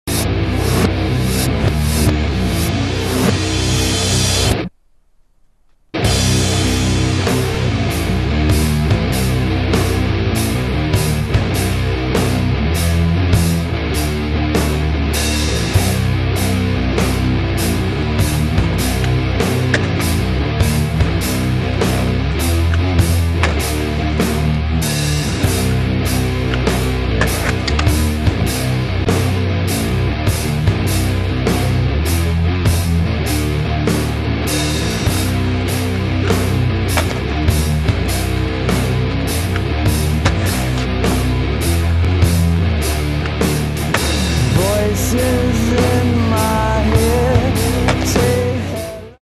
Dan Goodman vs. The ORR Nine.
Dan Goodman takes on the nine set at the high school.